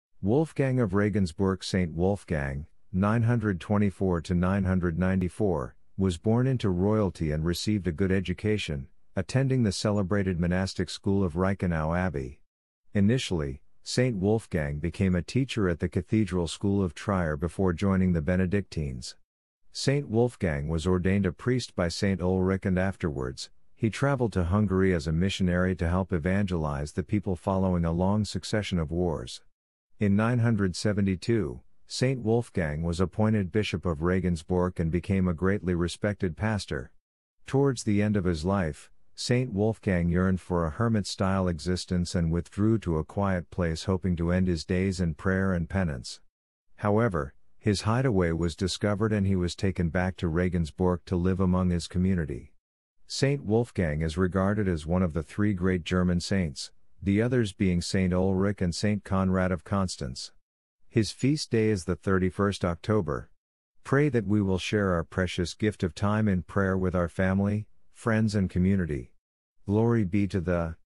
Prayers - Wolfgang of Regensburg
Wolfgang of Regensburg
 St Wolfgang (924-994) was born into Royalty and received a good education; attending the celebrated monastic school of Reichenau Abbey.
 Initially, St Wolfgang became a teacher at the Cathedral School of Trier before joining the Benedictines.
 St Wolfgang was ordained a Priest by St Ulrich and afterwards, he travelled to Hungary as a missionary to help evangelise the people following a long succession of wars. In 972, St Wolfgang was appointed Bishop of Regensburg and became a greatly respected Pastor.
 Towards the end of his life, St Wolfgang yearned for a hermit style existence and withdrew to a quiet place hoping to end his days in prayer and penance. However, his hideaway was discovered and he was taken back to Regensburg to live among his community.
 St Wolfgang is regarded as one of the three great German Saints; the others being St Ulrich and St Conrad of Constance. His Feast Day is the 31st October.
 Pray that we will share our precious gift of time in prayer with our family, friends and community.
 Glory be to the…